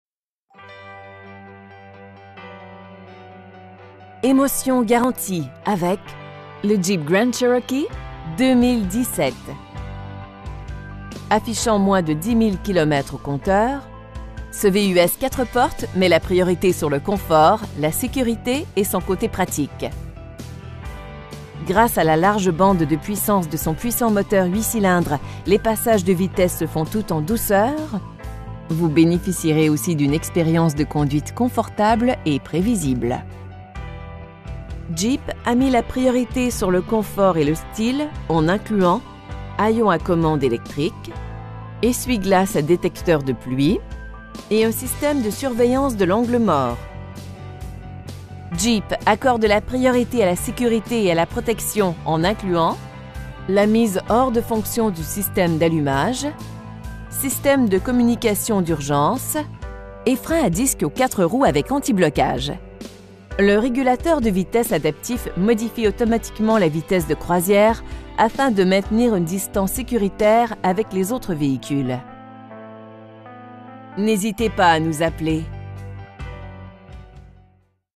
2017 Jeep Grand Cherokee SRT *$469*
L.A. Leasing
5625 Rue Paré
in Mont-Royal, QC H4P 1S1

Émotions garanties avec le Jeep Grand Cherokee 2017. Affichant moins de 10 000 km au compteur, ce VUS 4 portes met la priorité sur le confort, la sécurité et son côté pratique. Grâce à la large bande de puissance de son puissant moteur 8 cylindres, les passages de vitesses se font tout en douceur, vous bénéficierez aussi d'une expérience de conduite confortable et prévisible. Sa suspension bien calibrée et couplée à une système de contrôle de la stabilité procure un superbe agrément de conduite, tant en conduite dynamique que tranquille. Jeep a mis la priorité sur le confort et le style en incluant: des lampes de lecture avants et arrières, le siège conducteur à commande électrique, le rétroviseur central avec fonction antiéblouissement automatique, des rétroviseurs extérieurs avec fonction antiéblouissement automatique, hayon à commande électrique, essuie-glace à détecteur de pluie, et un système de surveillance de l'angle mort. Les passagers arrière seront reconnaissants de la présence de chauffe-siège en hiver. Comporte une télécommande intégrée d'ouvre-porte de garage, une autre des options distinctives de ce véhicule. Les garnissages de sièges de cuir de haute qualité ajoutent au confort, un luxe toujours apprécié du conducteur et des passagers. Avec des phares à haute intensité lumineuse pour éclairer la route, vous bénéficierez d'une visibilité optimale en toutes circonstances. Le système de navigation intégré procure des instructions à chaque intersection, et les commandes de chaîne stéréo au volant permettent d'aisément régler le système tout en conduisant. Jeep accorde la priorité à la sécurité et la protection en incluant: coussins gonflables à l'avant avec détecteur de poids du passager, rideaux gonflables pour protéger la tête, système antipatinage, système d'assistance au freinage, appuis-tête avant anti-coup de fouet, la mise hors de fonction du système d'allumage, système de communication d'urgence, et freins à disque aux 4 roues avec antiblocage. Le régulateur de vitesse adaptif modifie automatiquement la vitesse de croisière afin de maintenir une distances sécuritaires avec les autres véhicules. Nous offrons la plus agréable expérience d'achat, sans oublier les prix les plus concurrentiels. N'hésitez pas à nous appeler.